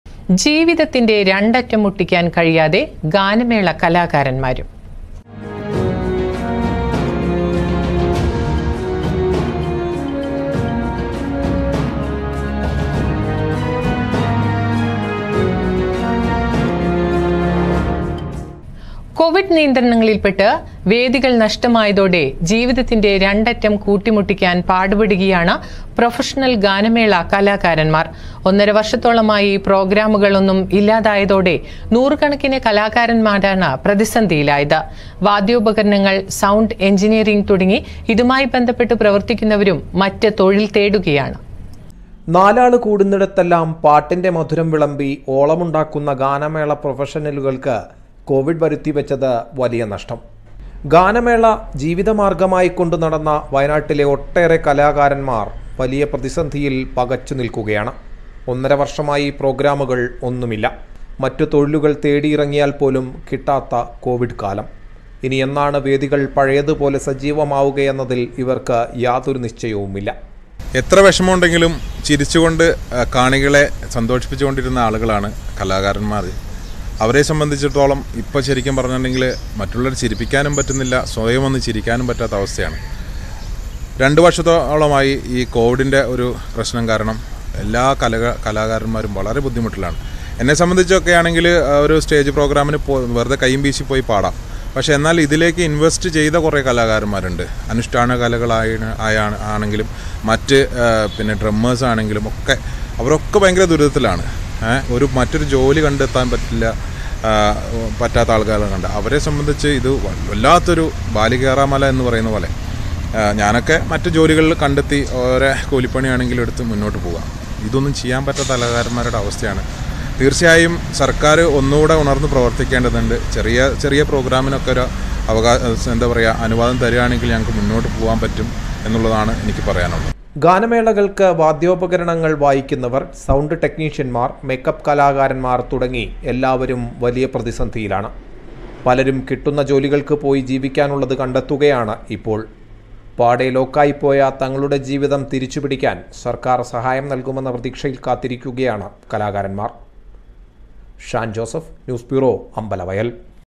MALANAD NEWS - 27-07-2021 - വേദികൾ നഷ്ടമായതോടെ പാട് പെടുകയാണ് പ്രൊഫഷണൽ ഗാനമേള കലാകാരൻമാർ.
കൊവിഡ് നിയന്ത്രണങ്ങളിൽപ്പെട്ട് വേദികൾ നഷ്ടമായതോടെ ജീവിതത്തിന്റെ രണ്ടറ്റം കൂട്ടിമുട്ടിക്കാൻ പാട് പെടുകയാണ് പ്രൊഫഷണൽ ഗാനമേള കലാകാരൻമാർ.